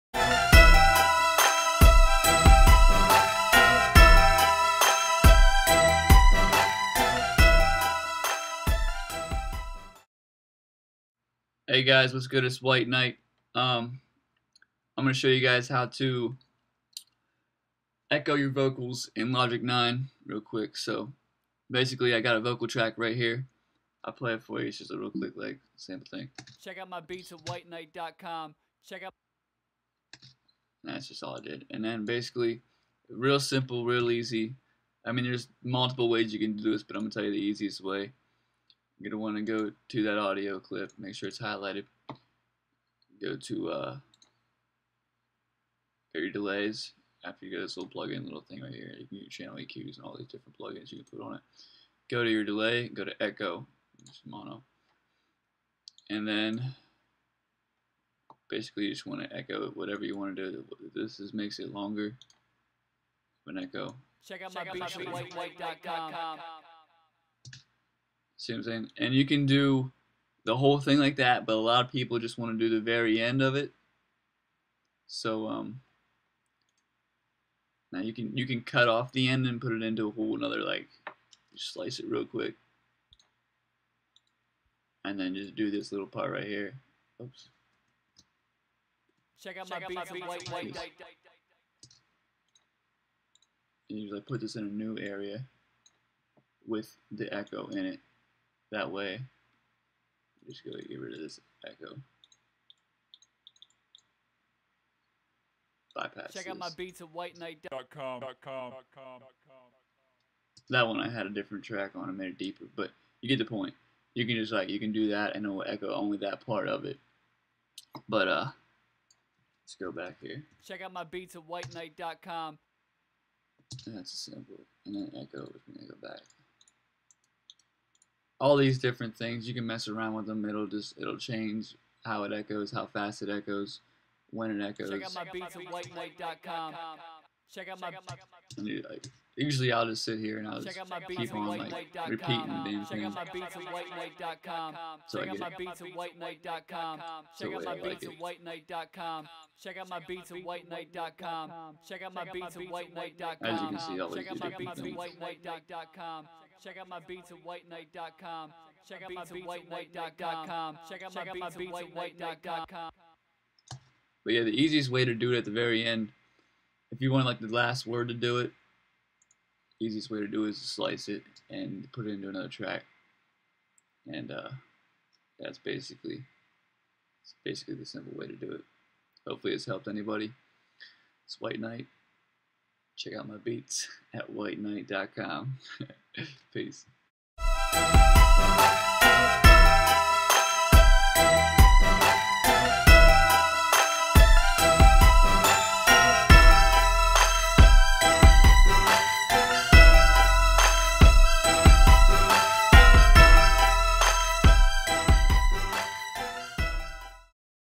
How To Put Echo On Vocal Track In Logic Pro 9 (simplest way)
This is just a quick tutorial video i did for anyone who is trying to find out how to echo vocal tracks in logic pro 9. I only showed the simplest and easiest way i believe to do it, but i know there are multiple other way to do this that can get a little more complicated. if you have any questions or want to see any certain other tutorial vids on logic 9 message me on here and ill get back to you!